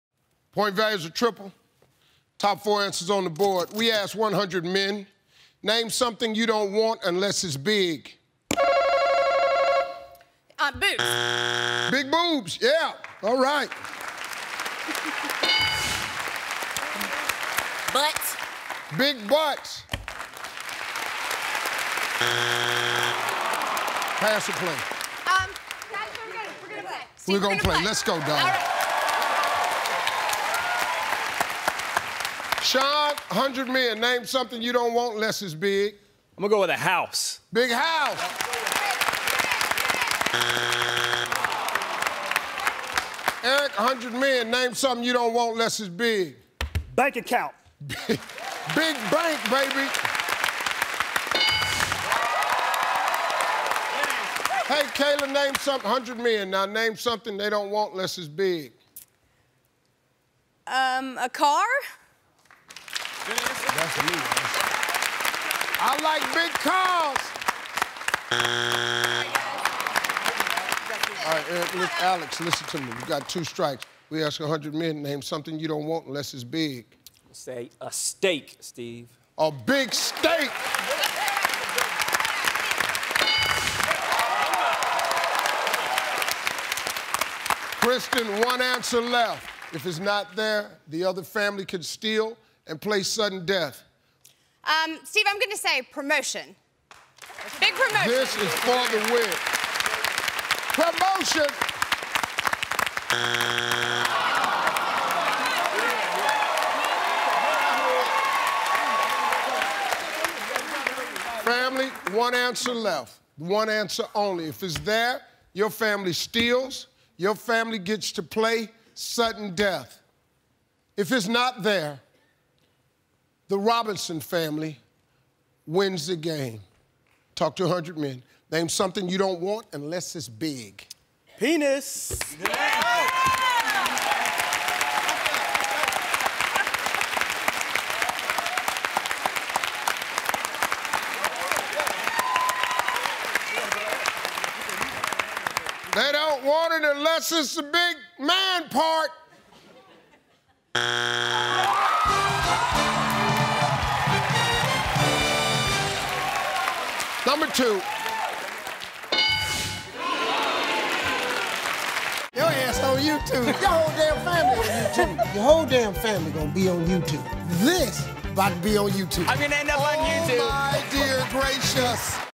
Guys don't want this unless it's big!
We asked 100 men... Name something you don't want unless it's big...